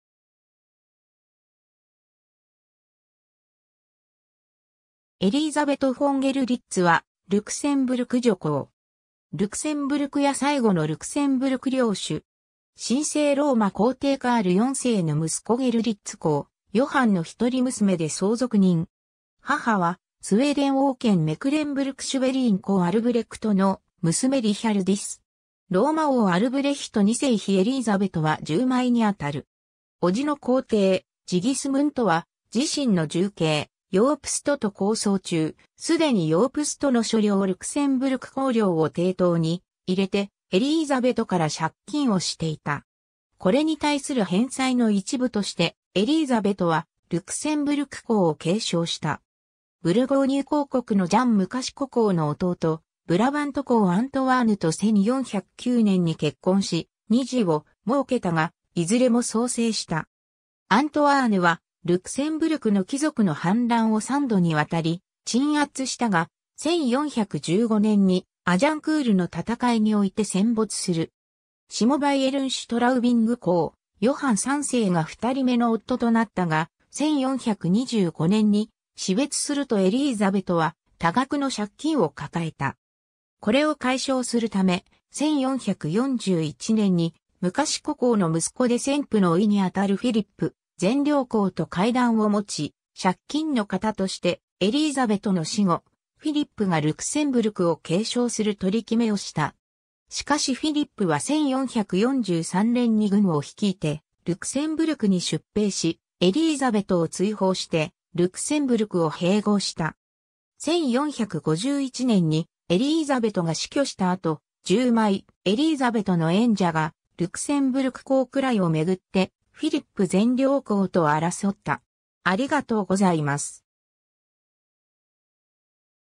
エリーザベト・フォン・ゲルリッツ
リンブルク＝ルクセンブルク家
エリーザベト・フォン・ゲルリッツ（Elisabeth von Görlitz, 1390年11月 - 1451年8月2日）は、ルクセンブルク女公（在位：1411年 - 1443年）。
ルクセンブルク家最後のルクセンブルク領主。
神聖ローマ皇帝カール4世の息子ゲルリッツ公ヨハンの一人娘で相続人。
母はスウェーデン王兼メクレンブルク＝シュヴェリーン公アルブレクト（アルブレヒト3世）の娘リヒャルディス。
ローマ王アルブレヒト2世妃エリーザベトは従妹にあたる。
 伯父の皇帝ジギスムントは自身の従兄ヨープストと抗争中、既にヨープストの所領ルクセンブルク公領を抵当に入れてエリーザベトから借金をしていた。
これに対する返済の一部として、エリーザベトはルクセンブルク公を継承した。
 ブルゴーニュ公国のジャン無畏公の弟、ブラバント公アントワーヌ（エリーザベトの大叔父に当たるルクセンブルク公ヴェンツェル1世の死後にブラバント公位を継いだ）と1409年に結婚し、2児を儲けたがいずれも早世した。
アントワーヌはルクセンブルクの貴族の反乱を3度にわたり鎮圧したが、1415年にアジャンクールの戦いにおいて戦没する。
 下バイエルン＝シュトラウビング公ヨハン3世が2人目の夫となったが、1425年に死別するとエリーザベトは多額の借金を抱えた。
これを解消するため、1441年に無畏公の息子で先夫の甥に当たるフィリップ善良公と会談を持ち、借金のかたとしてエリーザベトの死後、フィリップがルクセンブルクを継承する取り決めをした。
しかしフィリップは1443年に軍を率いてルクセンブルクに出兵し、エリーザベトを追放してルクセンブルクを併合した。
 1451年にエリーザベトが死去した後、従妹エリーザベトの縁者がルクセンブルク公位を巡ってフィリップ善良公と争った。